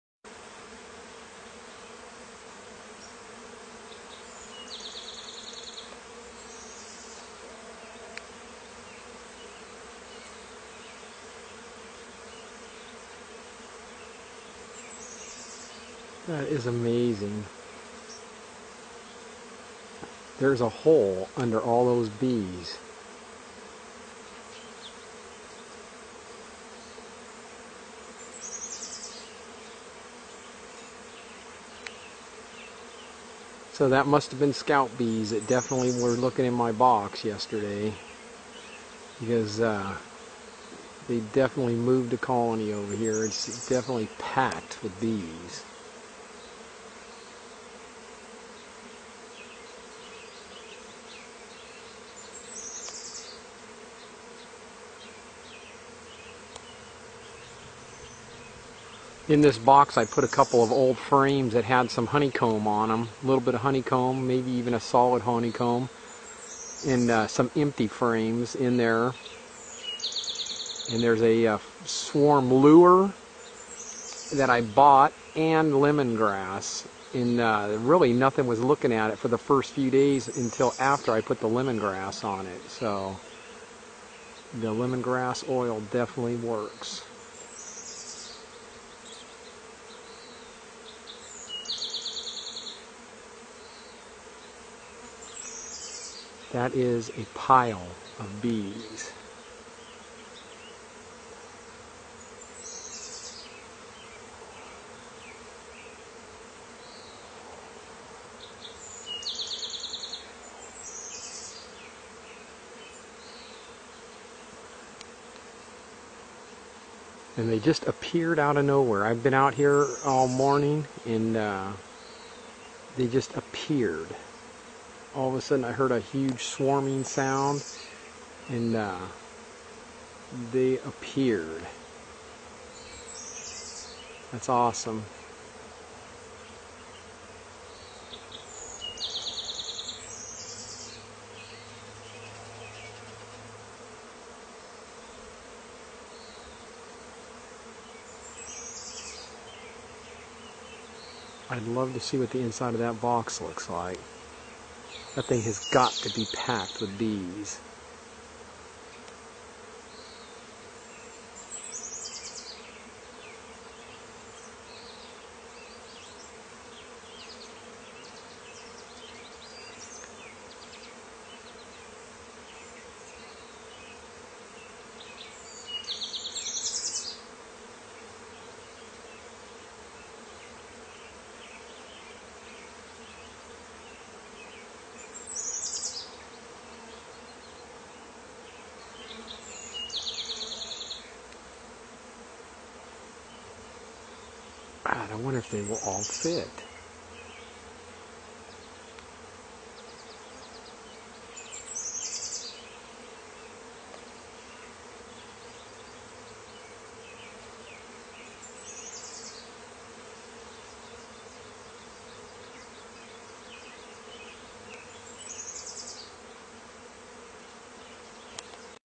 موسم التطريد عند النحل . نموذج لمصائد الطرود bitės Kyla
مع دخول شهر مارس يبدأ استعداد هواة ومحترفي المناحل لأول طرود النحل التي تخرج من الخلايا سواء بالمناحل المصنعة أو بالخلايا البرية.والفيديو يوضح كيف أعد هذا النحال مصيدة للطرود وكيف اختار المكان المناسب لنصبها. bitės Kyla